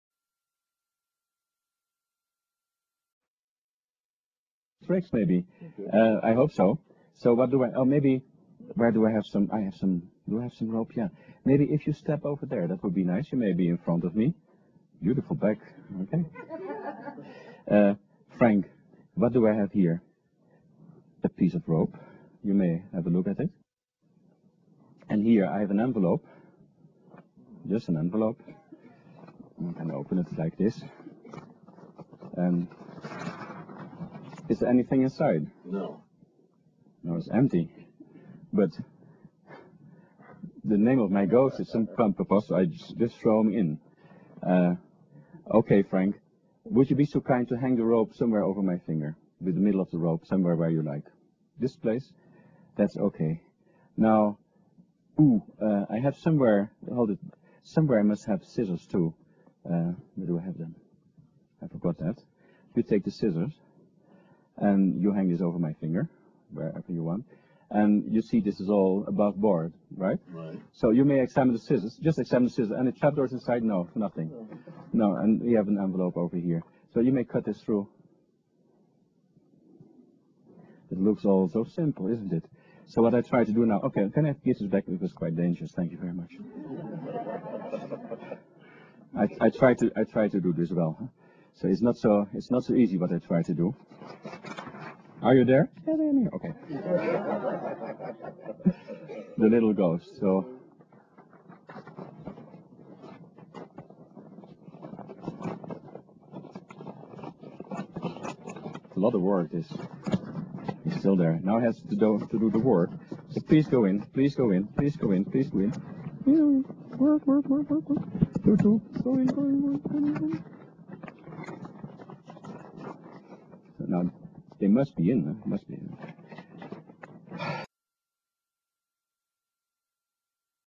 Cap Flotante Por Dirk Losander - Trick - asdetrebol.com
Recordamos que esta descripción ha sido traducida de forma automática por lo que puede contener erroresAutor: LosanderLosander, maestro del arte de la levitación, ha creado un nuevo efecto de levitación, El Cap flotante. Este efecto se puede realizar en un entorno de primer plano o en el escenario. Pedir prestado una gorra de béisbol, o cualquier sombrero que usted puede encontrar a partir de un miembro del público. También es necesario un pañuelo ordinario, que a su vez usted puede pedir prestado o producir a sí mismo. Simplemente cubra la tapa con el pañuelo y, de repente, sin explicación alguna, comienza a subir. La tapa vuela casi completamente cubierto de arriba abajo sin ningún movimiento de sus manos. Al final, las tierras de cabeza de nuevo en sus manos dejando a su público perplejo. Losander ha puesto sus más de 30 años de conocimiento en este sentido. Él ha creado un artilugio flotante que le permite flotar cualquier sombrero o gorra y sin tiempo de preparación. La tapa de flota como por arte de magia como la esfera, el cual fue creado por Losander y Tommy Wonder. Este paquete incluye un pañuelo, el truco y un DVD, que no sólo explica el efecto, lo que se puede aprender en un par de minutos, pero también le proporciona un valioso conocimiento de Losander sobre cómo mejorar sus habilidades de levitación ya existentes. El público creerá que usted es un verdadero mago!